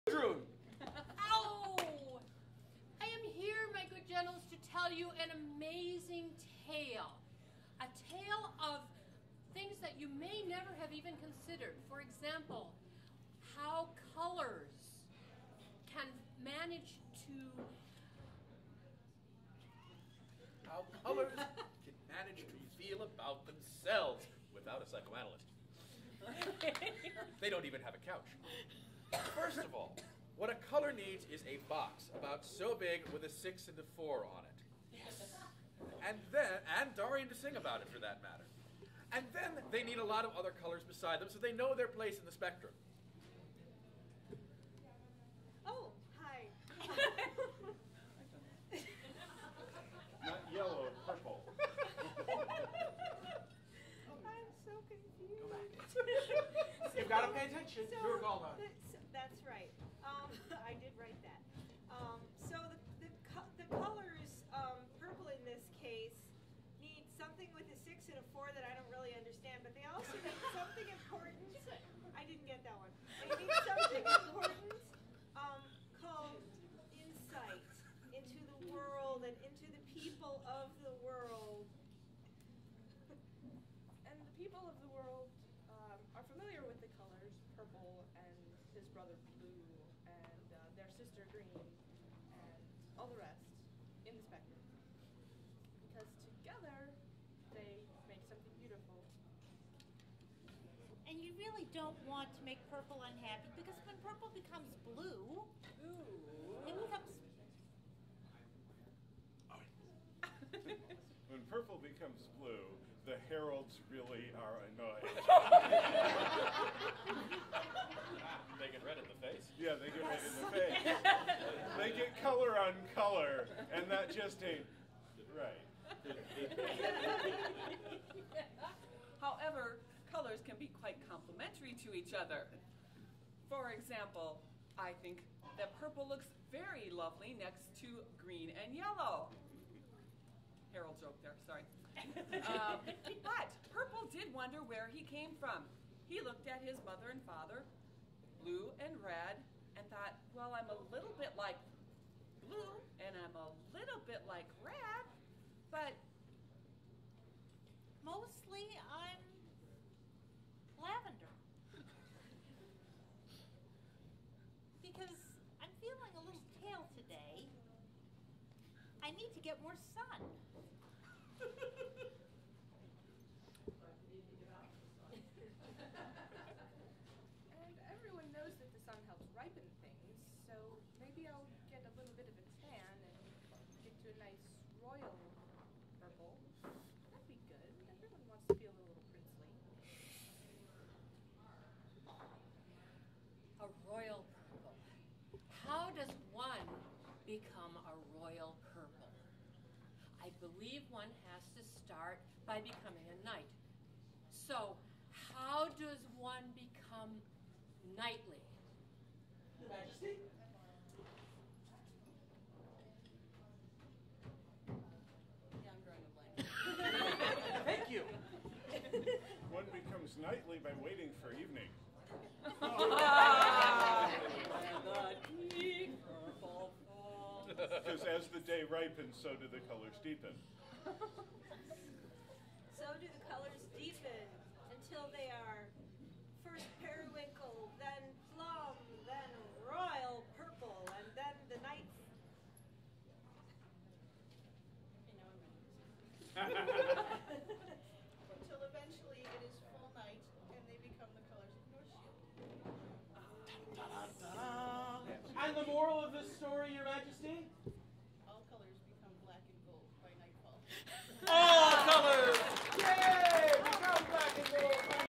Pass The Tale #2
The second "Past the Tale" challenge at Bardic Madness XX in the Barony of Windhaven on February 27, 2010. This challenge is an improv piece of sharing a story being directed by Dahrien Cordell.